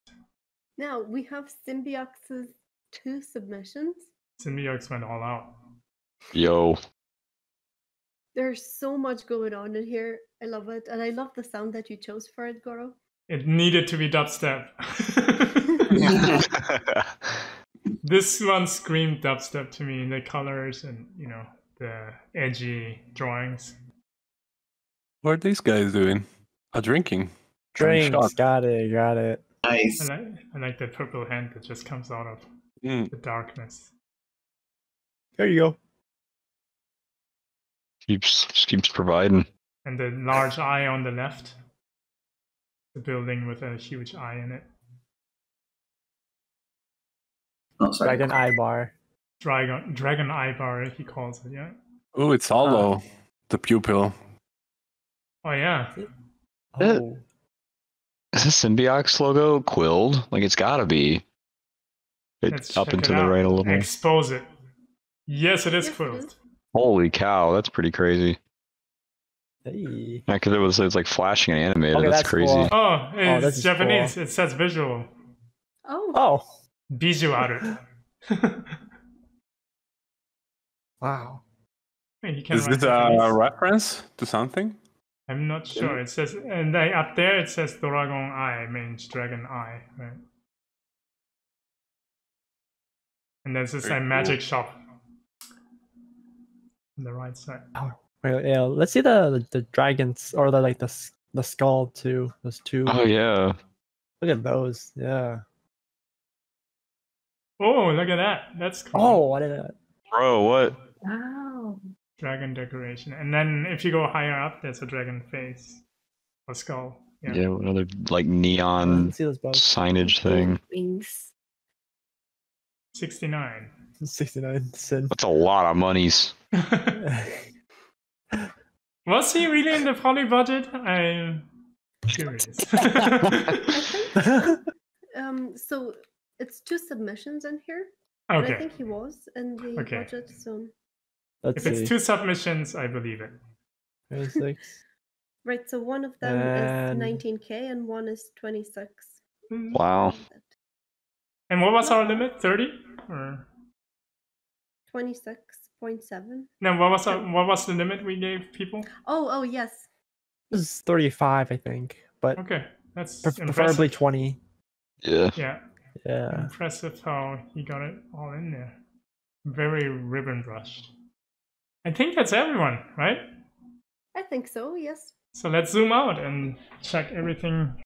Goro Fujita reviewing VR animation by SINBIOX for Virtual Animation CHINA TOWN HALLOWEEN
@gorofujita803  reviewing VR animation  by SINBIOX for Virtual Animation  CHINA TOWN HALLOWEEN